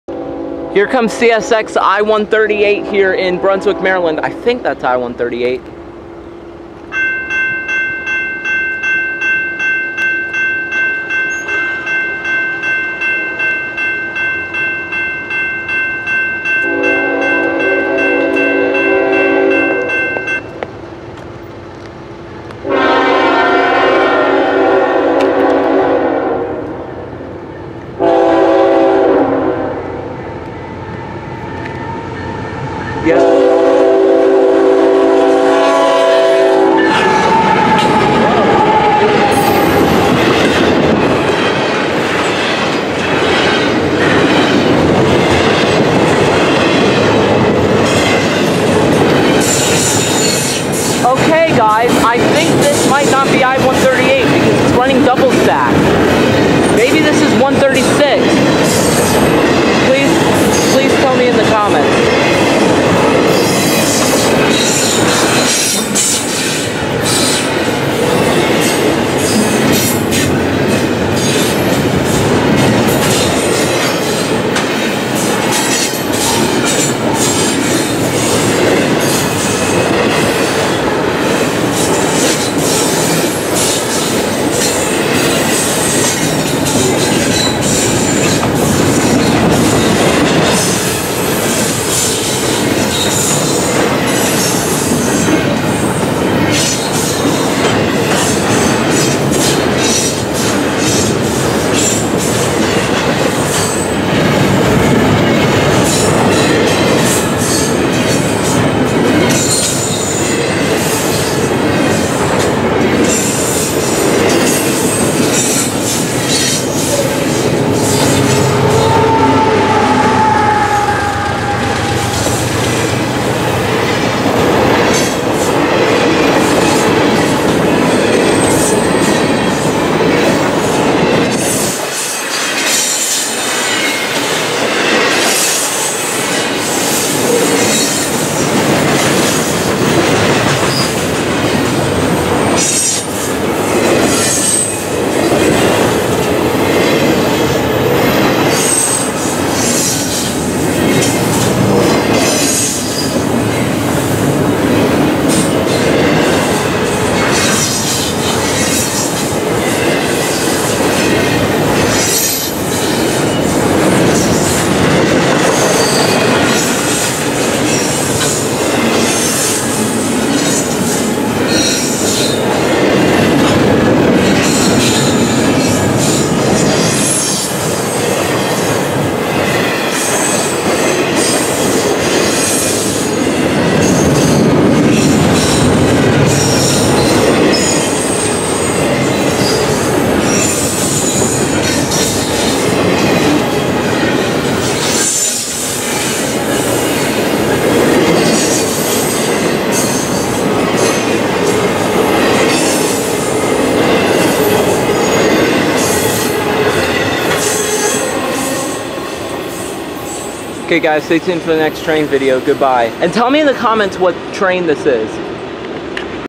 #655 CSX I136? at Brunswick Maryland
Taken July 10, 2022 I saw CSX I136? at Brunswick Maryland